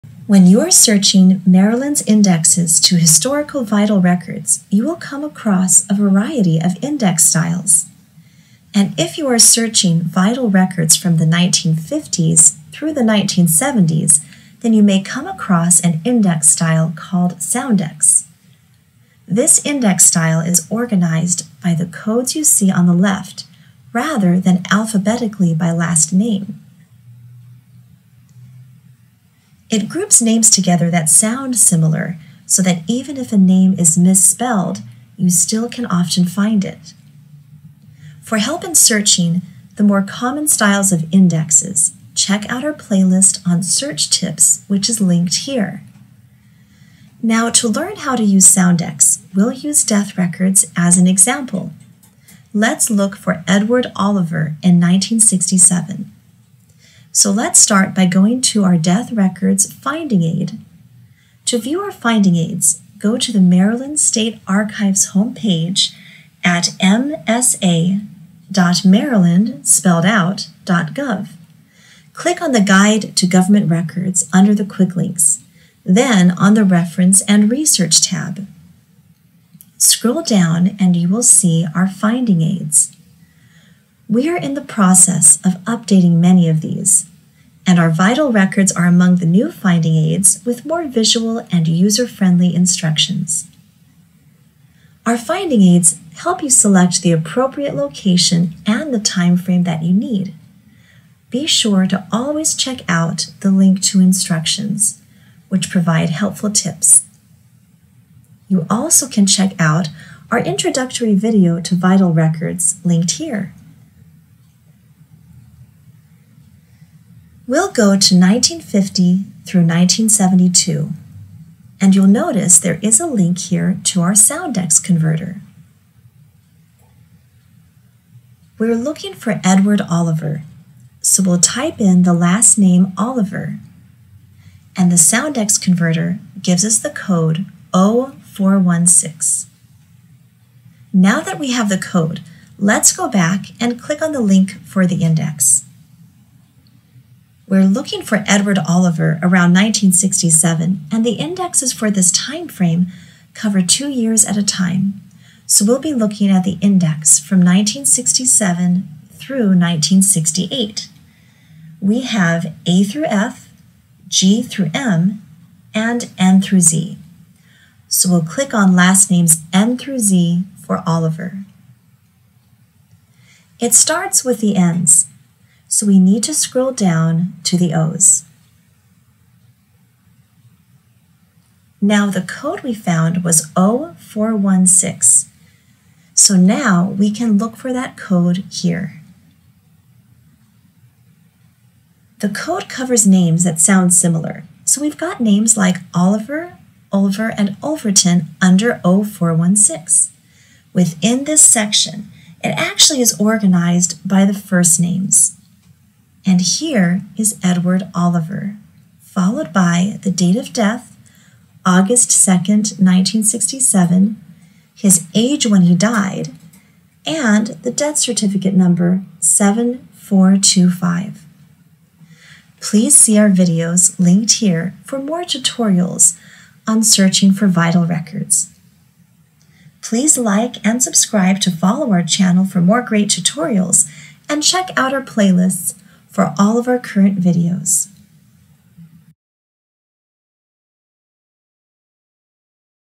Searching by Soundex
A brief tutorial on searching the historical Soundex index style.